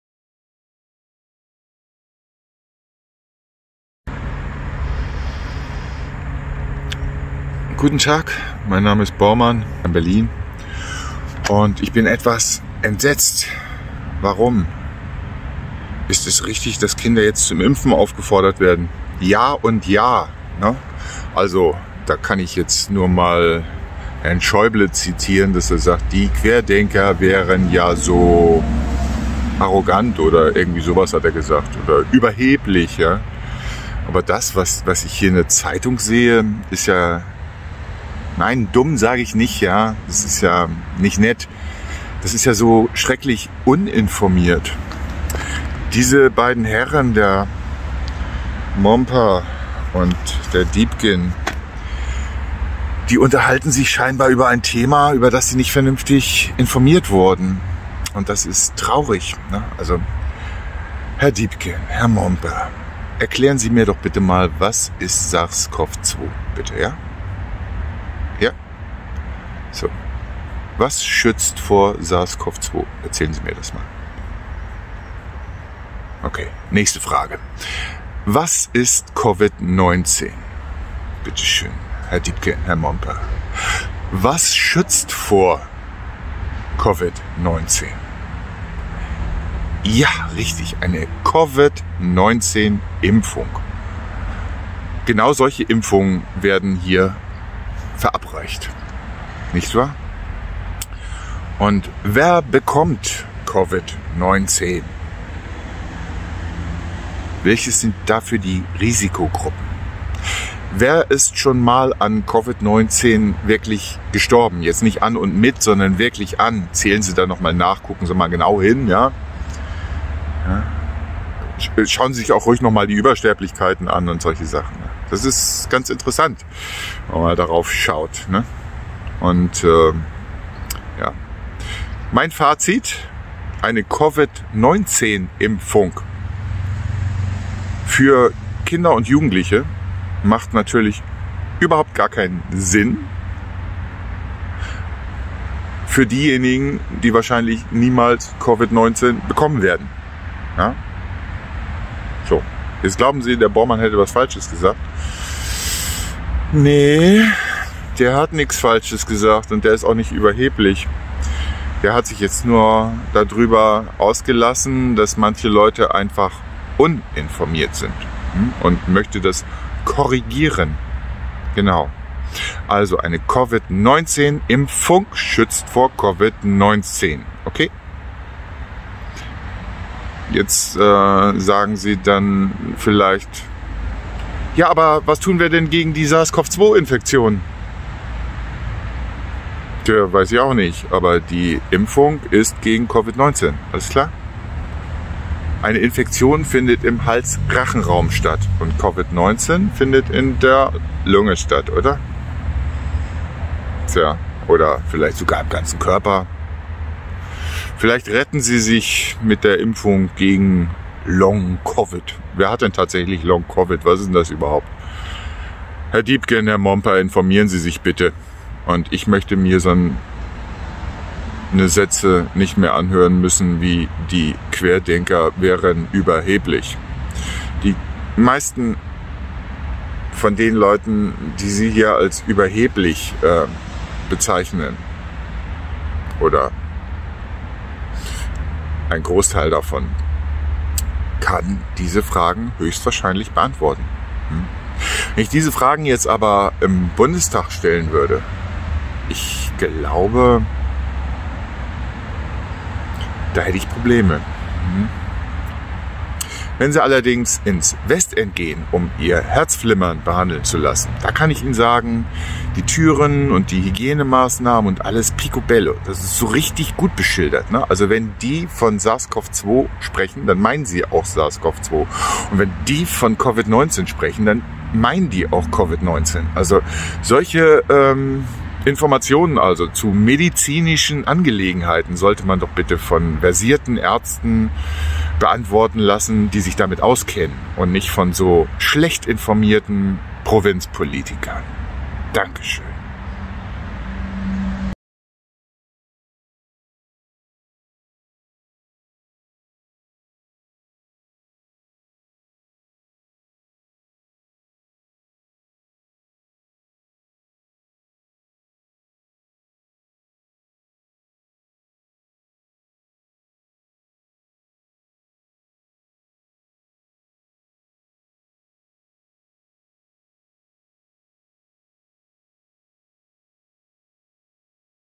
Uninformiert oder dummdreist?
Uninformiert scheinen sie zu sein - die Mompers und Diepgens dieser Welt! Was haben sie für ihren Zeitungsauftritt bekommen? Zuviel! Auch Herr Schäuble gibt die Wahrheit NICHT wider: »Wenn weltweit praktisch alle Fachleute sagen, Corona ist gefährlich und Impfen hilft, wer hat dann eigentlich das Recht zu sagen: Ich bin aber klüger? Das ist für mich ein nahezu unerträgliches Maß an Überheblichkeit«, sagte Schäuble der Neuen Osnabrücker Zeitung. Ich bin damit nicht einverstanden. Das ist so ziemlich das Dümmste, was ich lesen und mir anhören MUSS. Und alles andere wird vorsätzlich gelöscht! Schäuble hat mit seiner Aussage bewiesen, dass es ihm nicht um Wahrheit und offene Debatte geht, sondern um Besserwisserei! Der Mann ist unerträglich unvorsichtig! Niemand sollte unseren Kindern schlecht ausgetestete experimentelle Substanzen in den Körper spritzen! WER ÜBERNIMMT DAFÜR DIE HAFTUNG? WER KOMMT FÜR SCHÄDEN AUF? WARUM WIRD NICHT MEHR ÜBER IMPFSCHÄDEN BERICHTET?

Sofortige Rücktritt der Gesamten Bundesregierung wegen Amtsmissbrauch u. Hochverrat